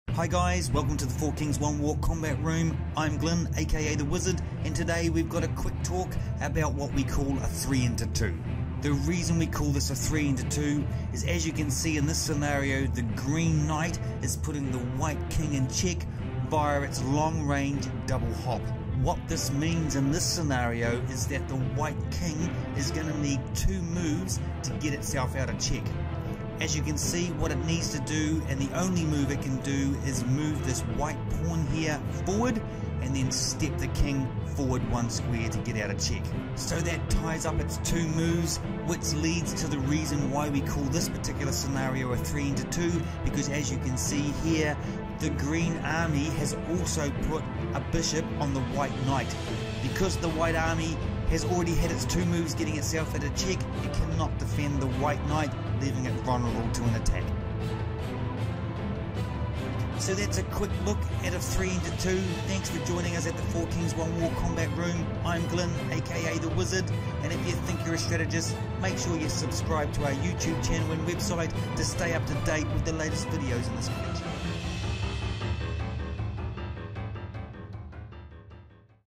Combat Room - Chess Strategy - Two move check (3 into 2)
Four Kings One War's style of chess strategy.  This is what we call a 3 into 2 via a two move check